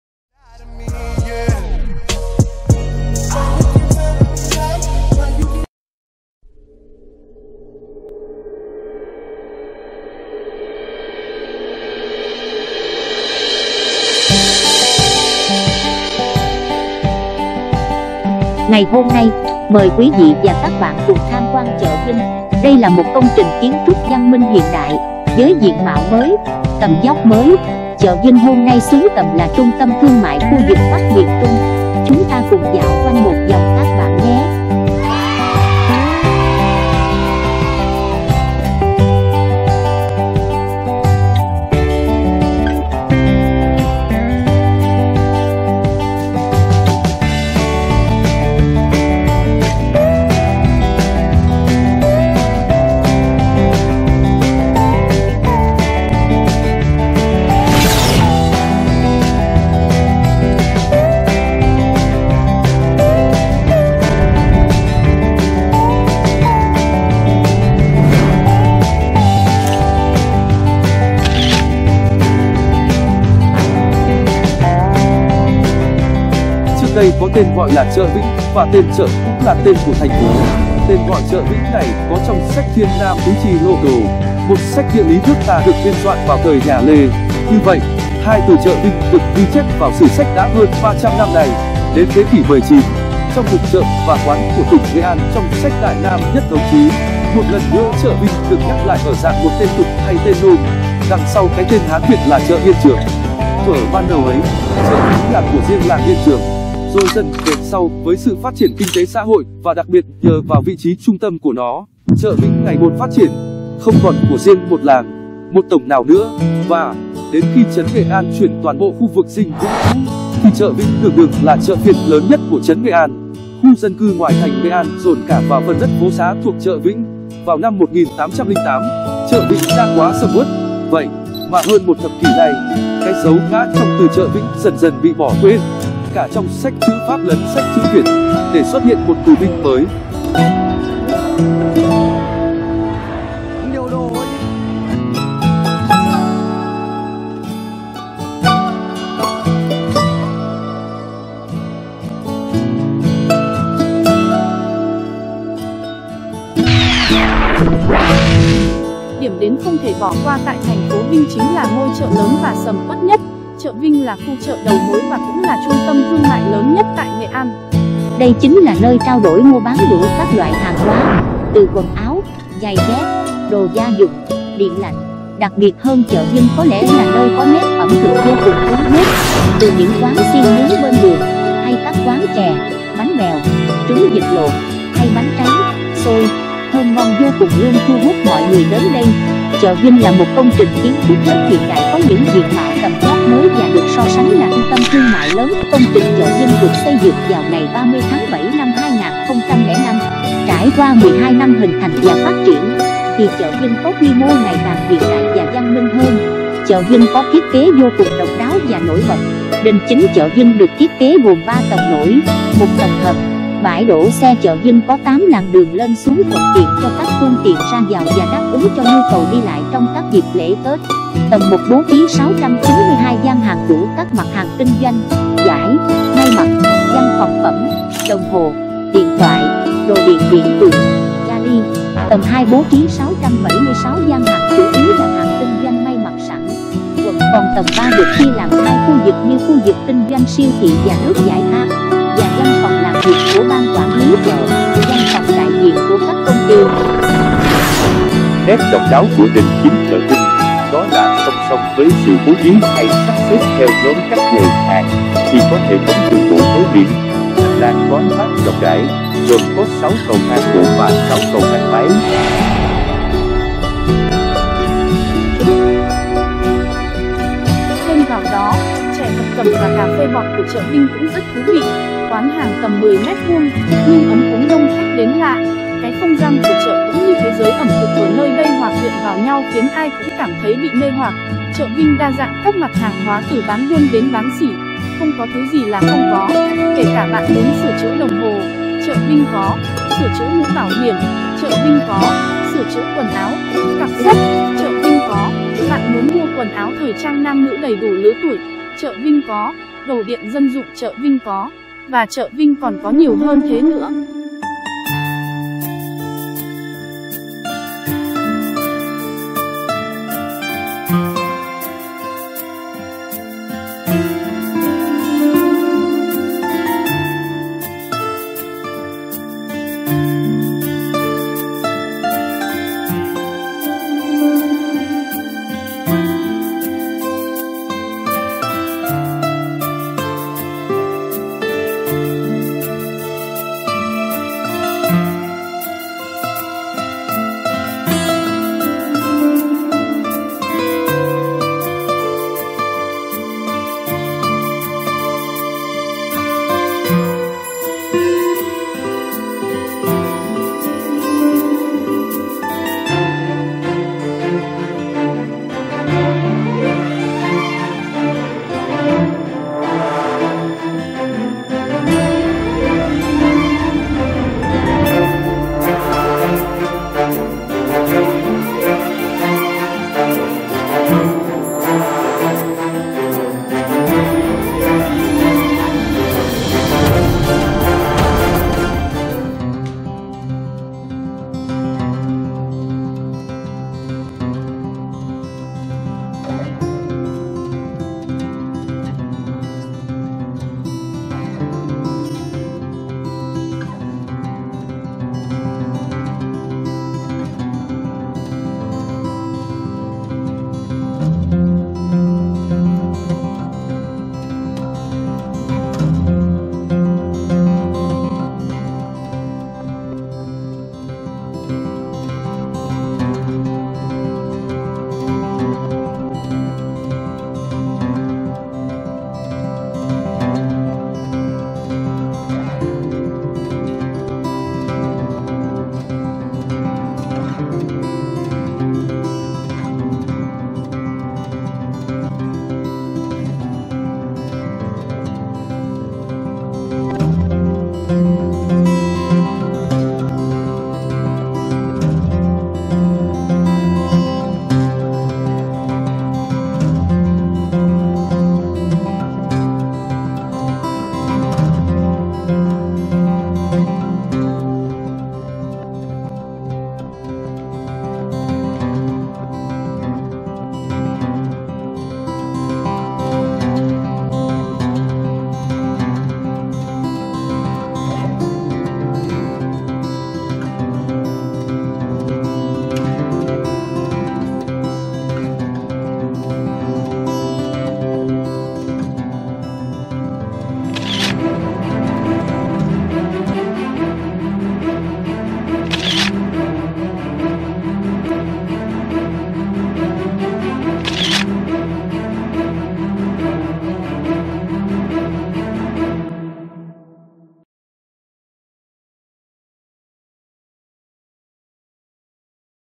Khám Phá Chợ Vinh - Trung Tâm Thương Mại Lớn Nhất Bắc Miền Trung
Du Lịch Nghệ An Khám Phá Chợ Vinh - Trung Tâm Thương Mại Lớn Nhất Bắc Miền Trung , Đây là một công trình kiến trúc văn minh hiện đại, với diện mạo mới, tầm vóc mới.
Rất mong được sự ủng hộ từ các bạn
© Bản quyền thuộc về Du lich kham pha 9XStudio
© Copyright by 9XStudio Production ☞ Do not Reup
Chợ Vinh - trước đây có tên gọi là chợ Vĩnh và tên chợ cũng là tên của Thành phố. Tên gọi chợ Vĩnh này có trong sách "Thiên nam tứ chi lộ đồ", một sách địa lý nước ta được biên soạn vào thời nhà Lê. Như vậy, hai từ chợ Vinh được ghi chép vào sử sách đã hơn 300 năm nay.
Đến thế kỷ XIX, trong mục chợ và quán của tỉnh Nghệ An trong sách Đại nam nhất thống chí, một lần nữa chợ Vinh được nhắc lại ở dạng một tên tục hay tên nôm, đằng sau cái tên Hán Việt là chợ Yên Trường. Thuở ban đầu ấy, chợ Vĩnh là của riêng làng Yên Trường. Rồi dần về sau với sự phát triển KTXH và đặc biệt nhờ vào vị trí trung tâm của nó, chợ Vĩnh ngày một phát triển, không còn của riêng một làng, một tổng nào nữa. Và, đến khi trấn Nghệ an chuyển toàn bộ khu vực dinh Vĩnh cũ, thì chợ Vĩnh đường đường là chợ phiên lớn nhất của trấn Nghệ An, khu dân cư ngoài thành Nghệ An dồn cả vào phần đất phố xá thuộc chợ Vĩnh. Vào năm 1808, chợ Vĩnh đã quá sầm uất. Vậy ,mà hơn một thập kỷ nay, cái dấu ngã trong từ chợ Vĩnh dần dần bị bỏ quên, cả trong sách chữ Pháp lấn sách chữ Việt, để xuất hiện một từ "Vinh" mới.